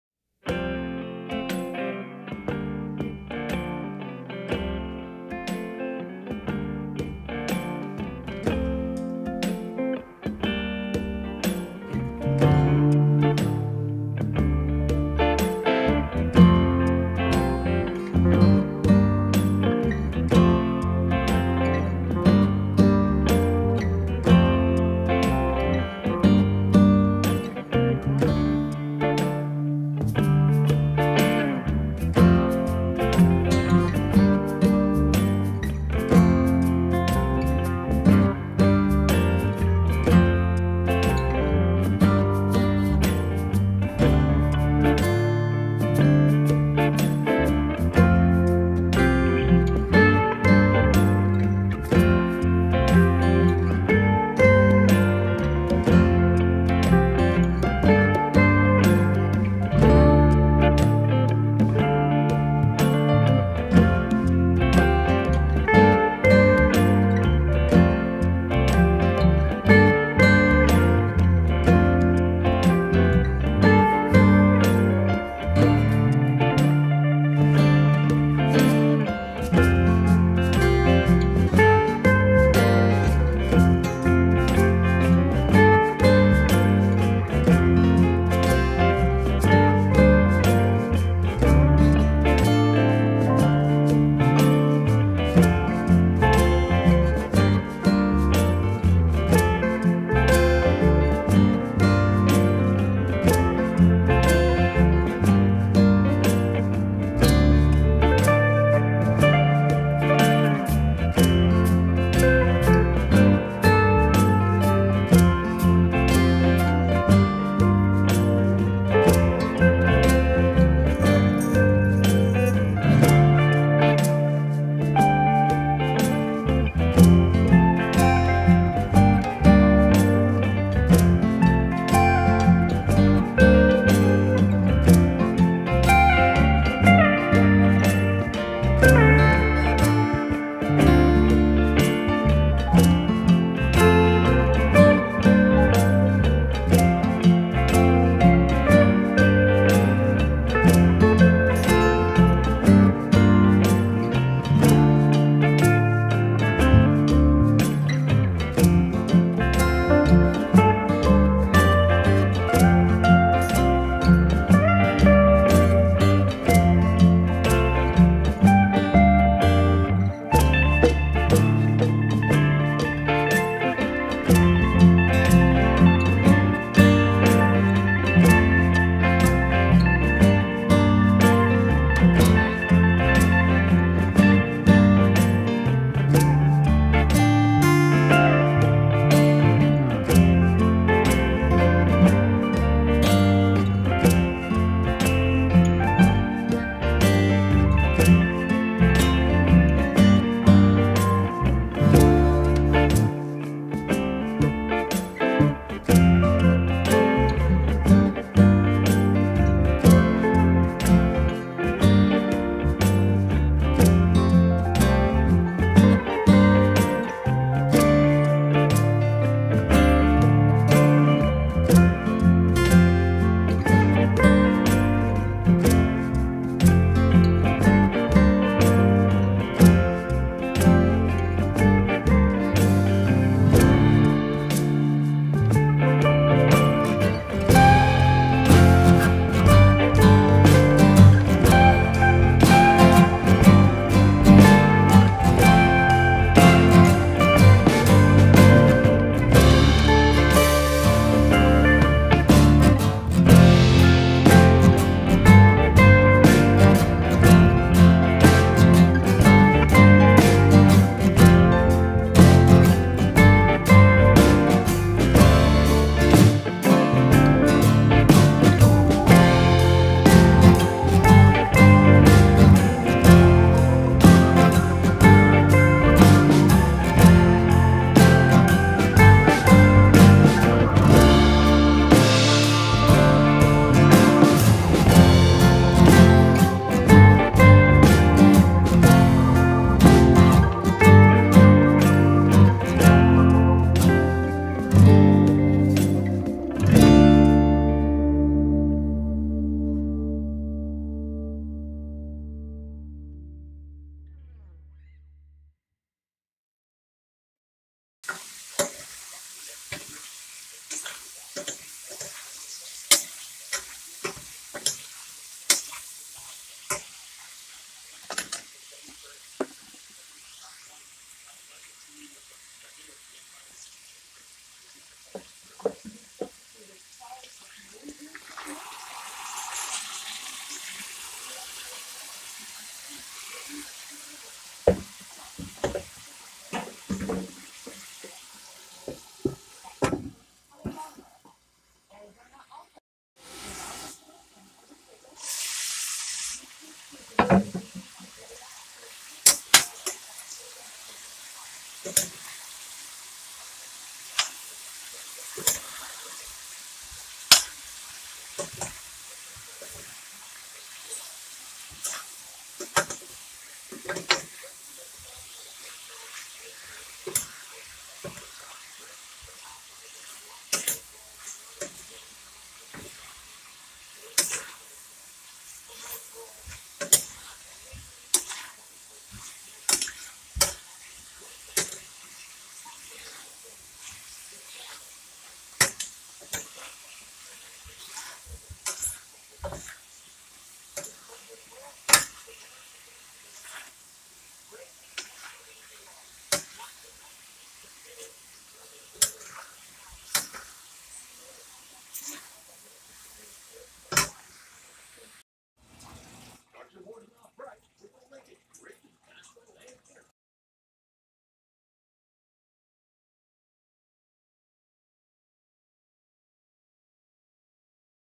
Chayote Squash with Tofu and Mung Bean Sprout Sauteed in Pork Belly Fat./Baakjimiuh47
Chayote squash with tofu and mung bean sprout sauteed in pork bell fat season with black pepper, soy sauce, hot sauce and chicken powder.